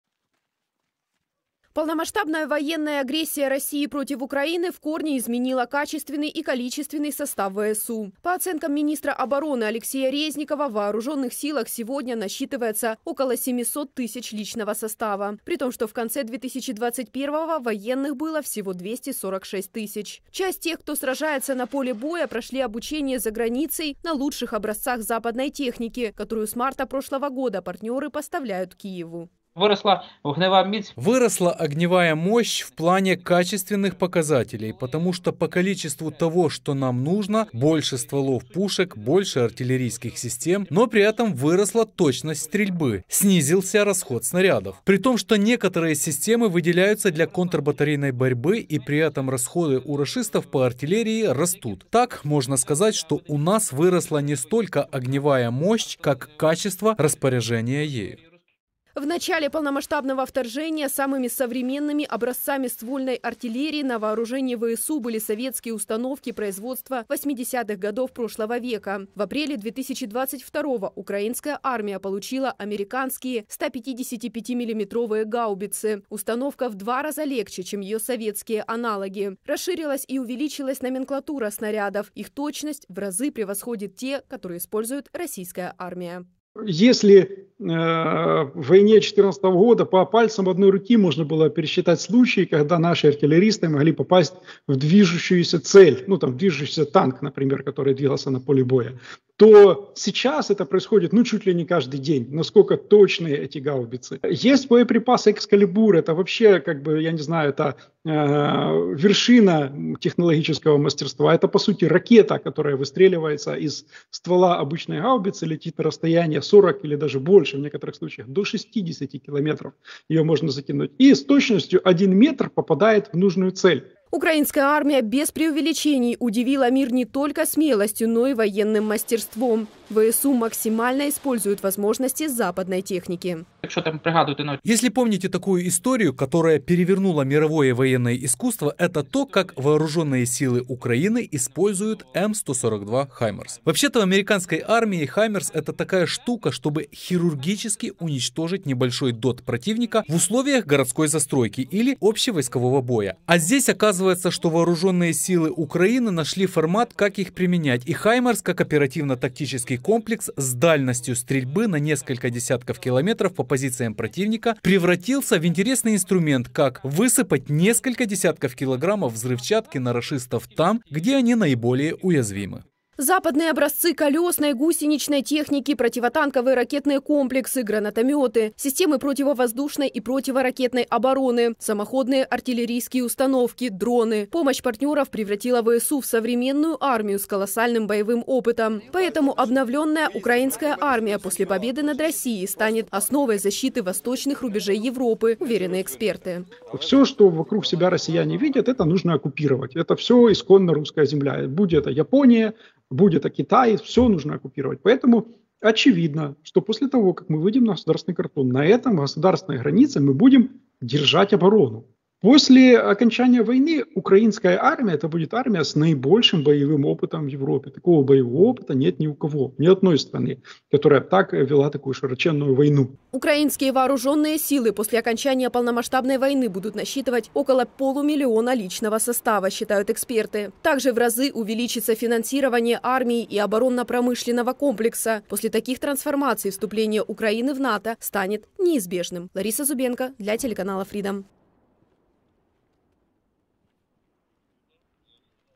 Укрепление сил ВСУ. Трансформация армии Украины за год полномасштабной войны
Украина поднялась на 15 место в рейтинге самых сильных армий мира. Такие данные приводят аналитики сайта Global Fire Power. При этом в прошлом году ВСУ занимали 22 позицию среди армий 145 государств. Как отмечают авторы, война против Украины продемонстрировала военные ограничения России, несмотря на ее превосходящую в количестве живую силу. Детальнее о том, как за более чем год полномасштабного вторжения трансформировалась украинская армия, смотрите в материале